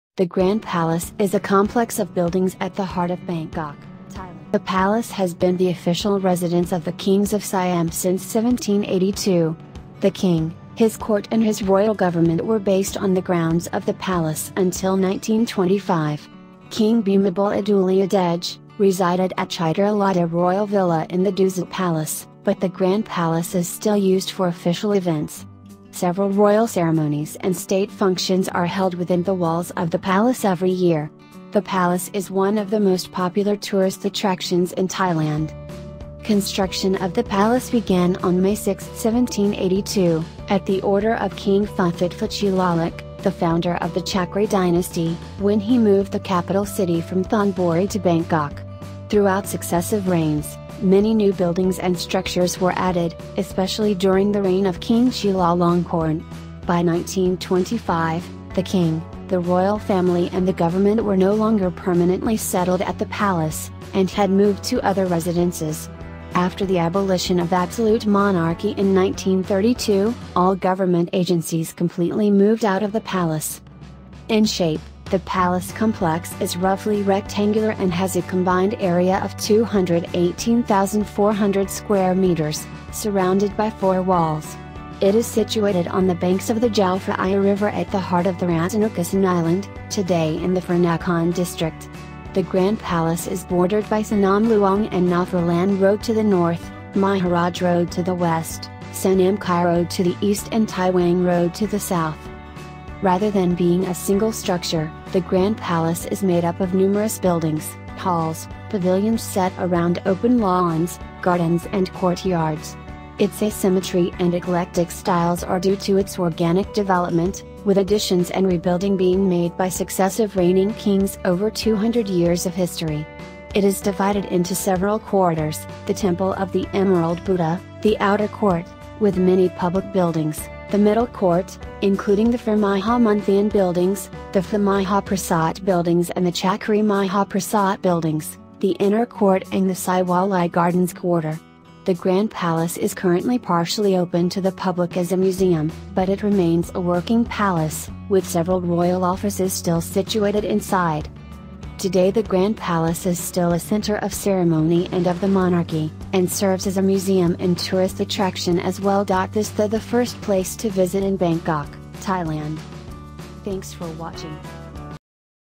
The Grand Palace in Bangkok ( Thailand )
The Grand Palace

If there is one must-see sight that no visit to Bangkok would be complete without, it's the dazzling, spectacular Grand Palace, undoubtedly the city's most famous landmark. Built in 1782 - and for 150 years the home of the Thai King, the Royal court and the administrative seat of government - the Grand Palace of Bangkok is a grand old dame indeed, that continues to have visitors in awe with its beautiful architecture and intricate detail, all of which is a proud salute to the creativity and craftsmanship of Thai people. Within its walls were also the Thai war ministry, state departments, and even the mint. Today, the complex remains the spiritual heart of the Thai Kingdom.

Opening Hours: Daily 08:30 - 15:30 Location: Na Phra Lan Road, Old City (Rattanakosin) Price Range: Tickets sold from 8:30 - 15:30 and cost 500 baht! One ticket includes entry to Vimanmek Palace and Abhisek Dusit Throne Hall